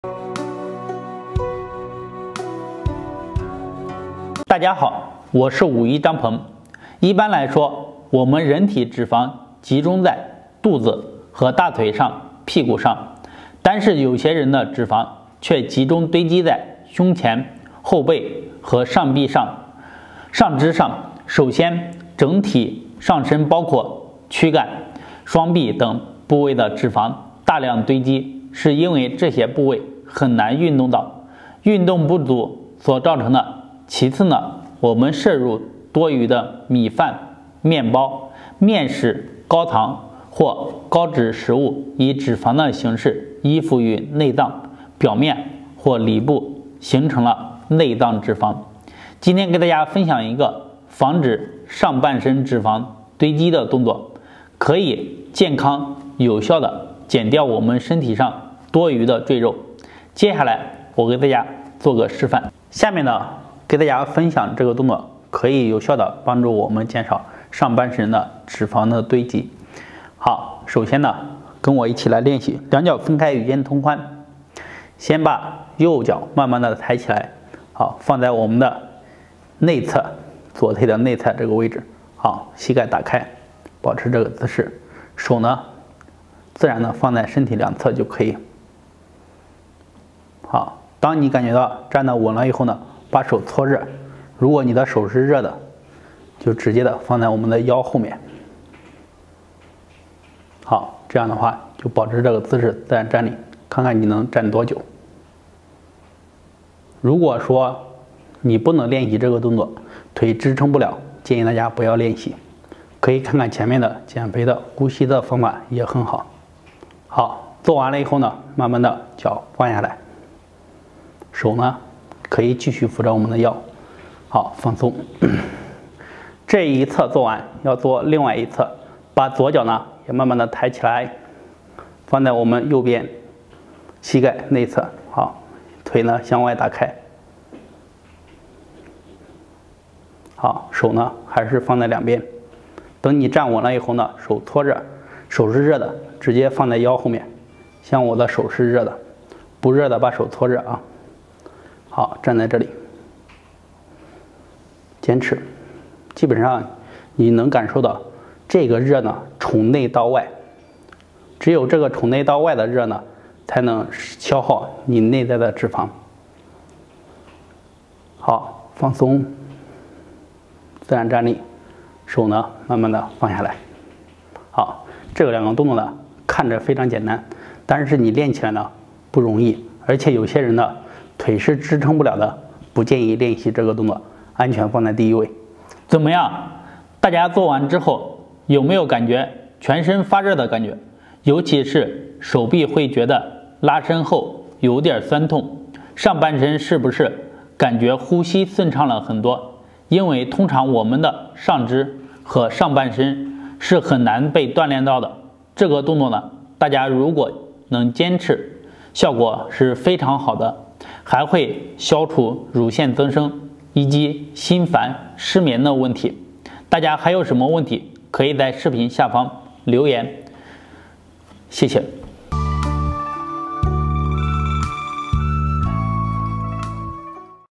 上半身脂肪堆積，虎背熊腰，每天站樁3分鐘，拉伸全身，減去贅肉【武醫張鵬養生】
本頻道製作的所有影片都是原創拍攝的作品，所有拍攝過程都保證了安全。 人體脂肪主要集中在肚子以及腿上，但有些人則是上半身脂肪堆積，今天給大家分享一個防止上半身脂肪堆積的動作，每天站樁3分鐘，拉伸全身，減去贅肉~